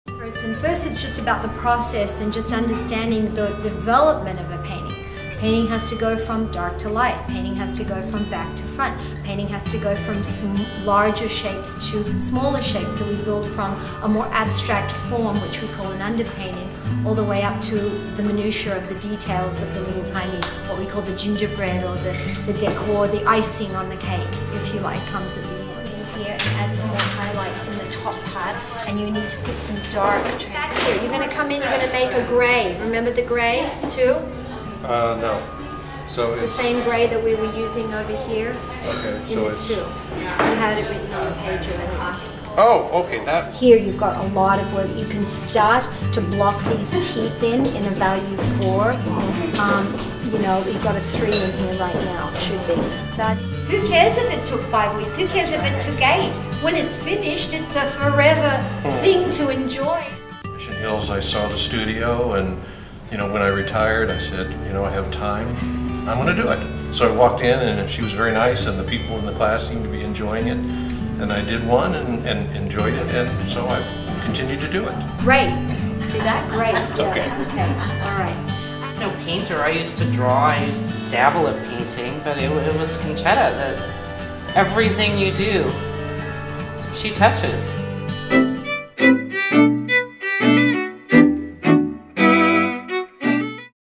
The Salon of Art - Oil Painting Classes San Diego
The Salon of Art in San Diego's historic Mission Hills, offers an array of educational art classes for beginning to advanced students. Oil painting instruction is offered by master artist Concetta K. Antico.Her unique approach to teaching her students how to oil paint is straight forward, challenging and highly rewarding.